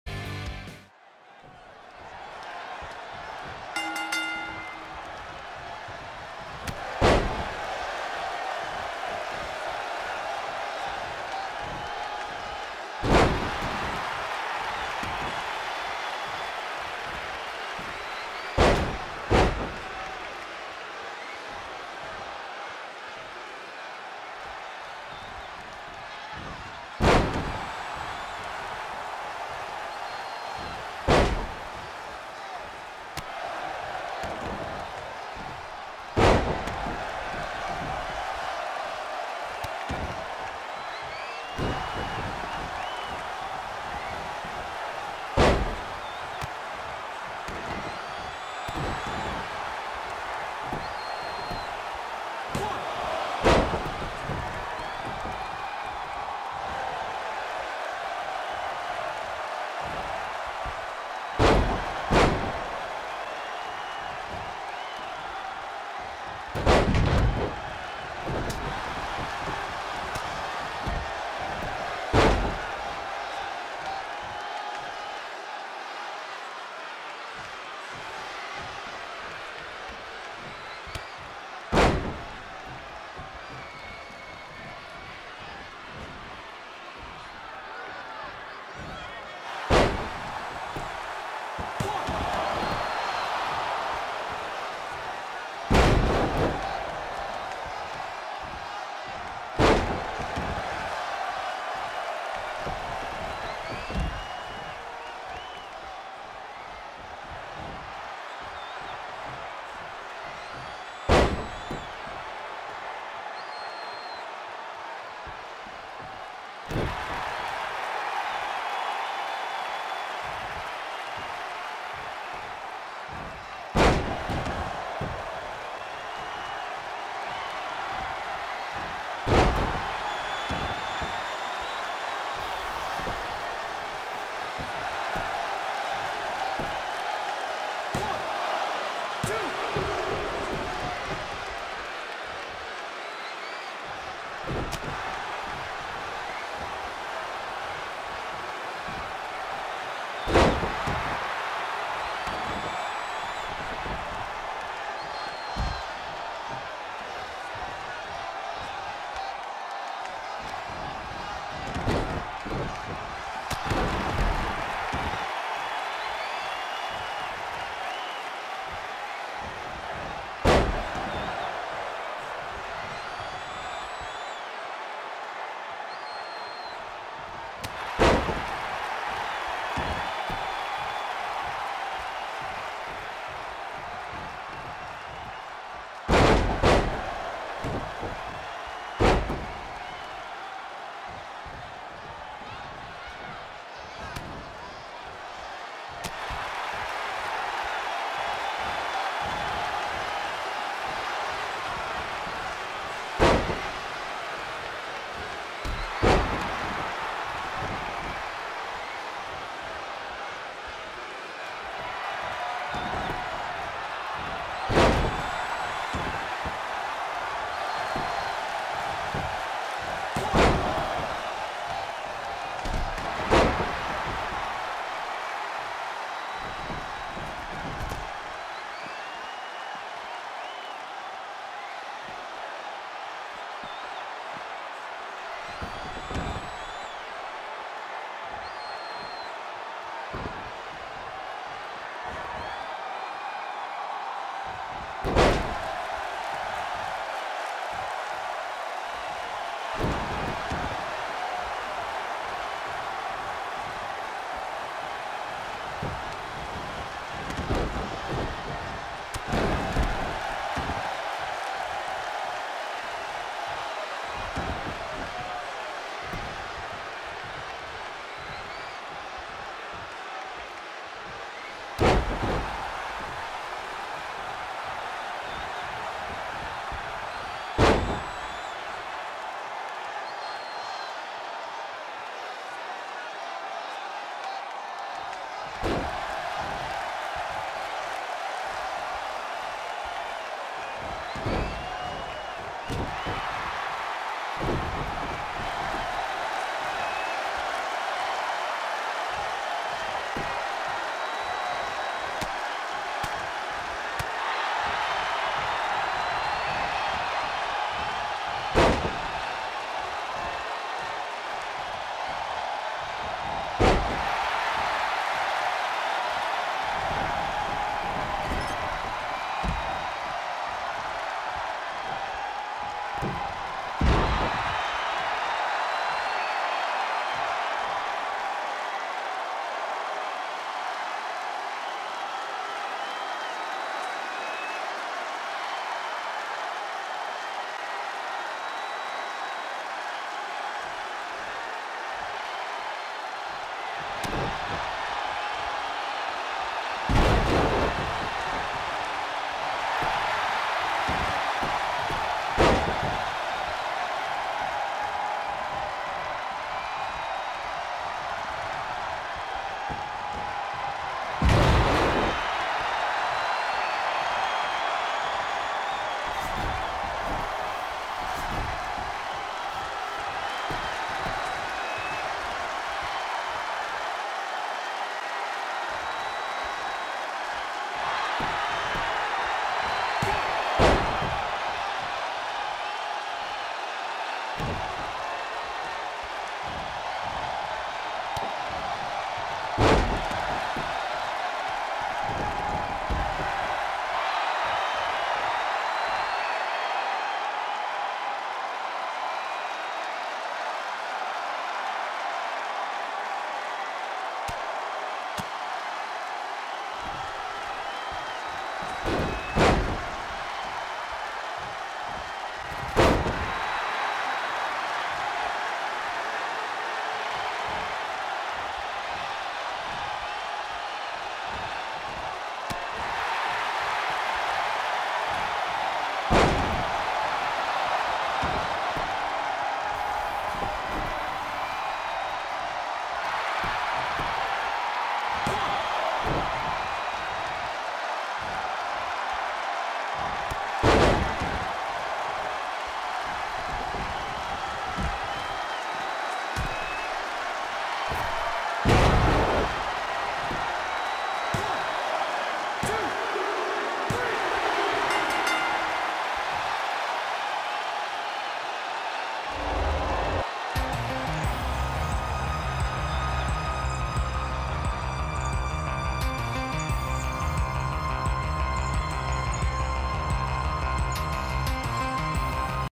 TCW-Carnage-01
We start out our first TCW Carnage of 2022 with Mr. Anderson VS Warcry VS Skye.